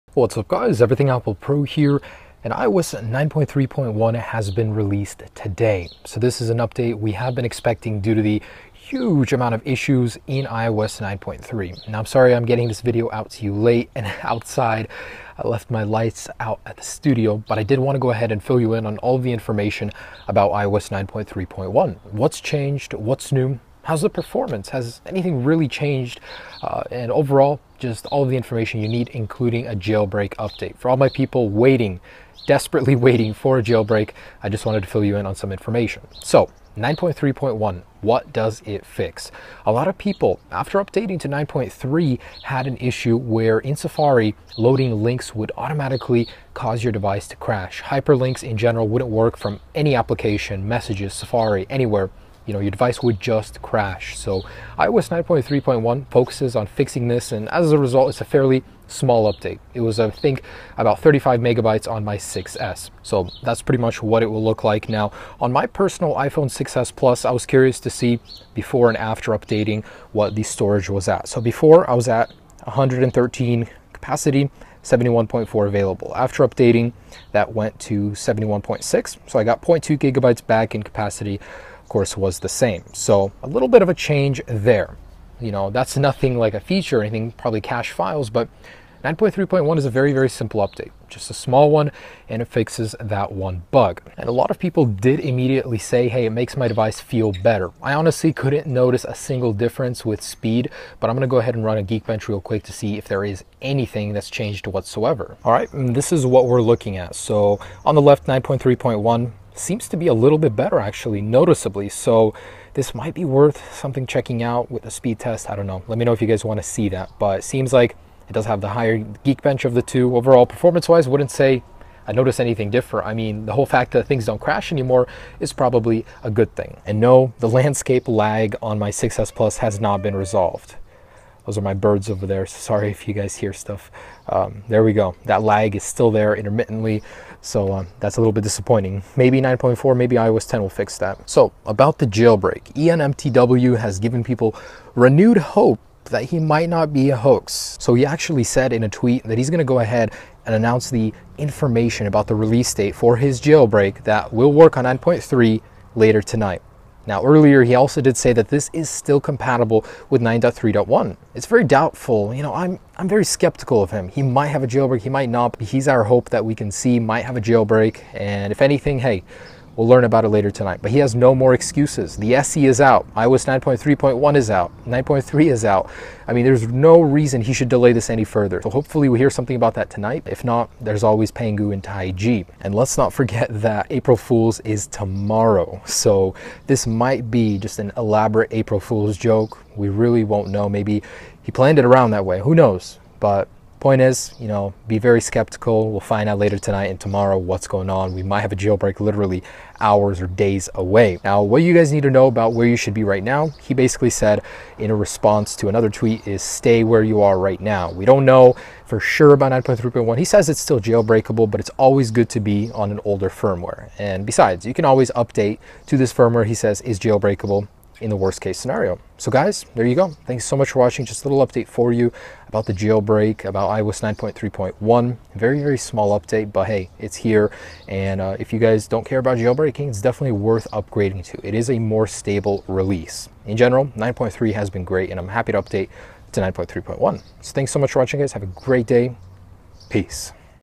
iOS 9.3.1 Released - Everything You Need To Know!
iOS 9.3.1 Is Out! New Features, Performance & Jailbreak Update! Is iOS 9.3.1 vs iOS 9.3 Any Faster? When�۪s The Jailbreak Release Date??